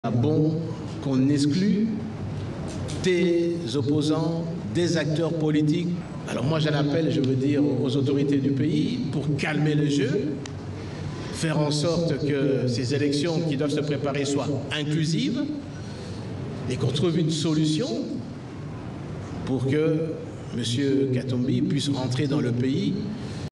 L'ancien vice-président Jean-Pierre Bemba plaide pour le retour de Katumbi.
L'ancien vice-président Jean-Pierre Bemba, qui lui a pu rentrer ce mercredi en RDC plaide pour le retour de  Katumbi.